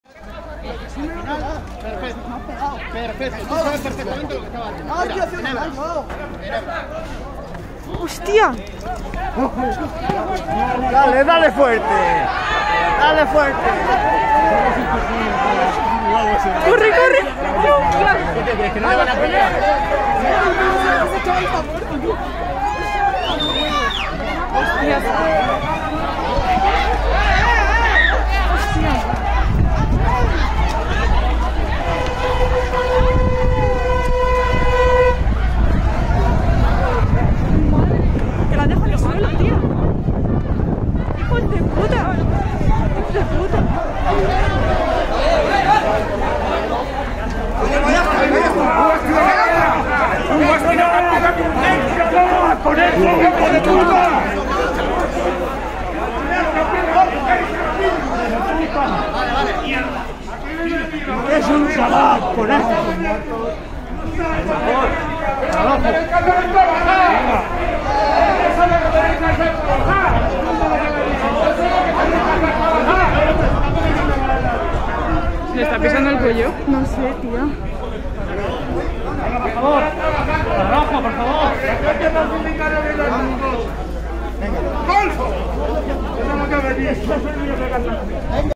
agresión GC localidad Navalcarnero madrid
Video doméstico sobre la agresión sufrida por un GC -de paisano-  en la localidad de Navalcarnero.
En el contexto de un mitin políco.